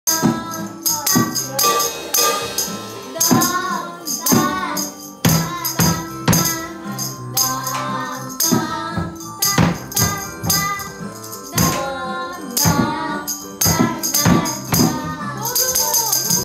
福井県越前市Toko toko教室 発達障がい児音楽レッスン
福井県越前市でToko toko教室をしている、田中さとこです。発達障がい児の生徒さんのレッスンの様子です。